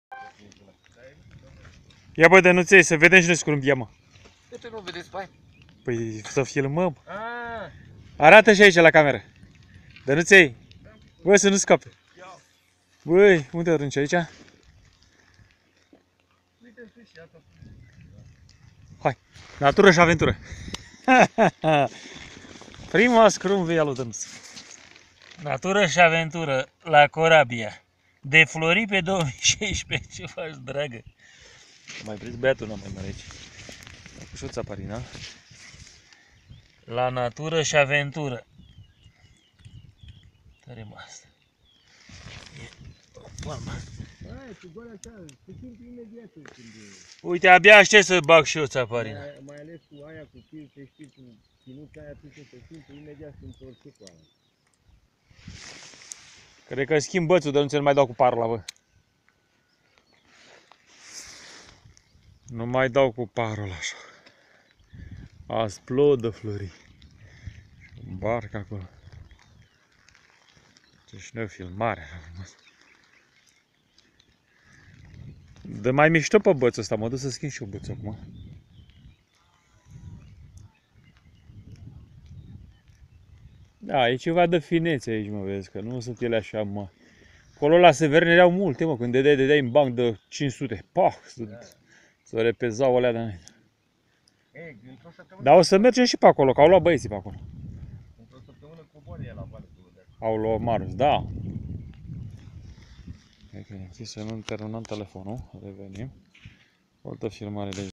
PESCUIT SCRUMBIE LA CORABIA 24-04-2016
CAM SLABUT,DOAR 4 SCRUMBII A PRINS PRIETENUL MEU DAN,EU ,DATA VIITOARE.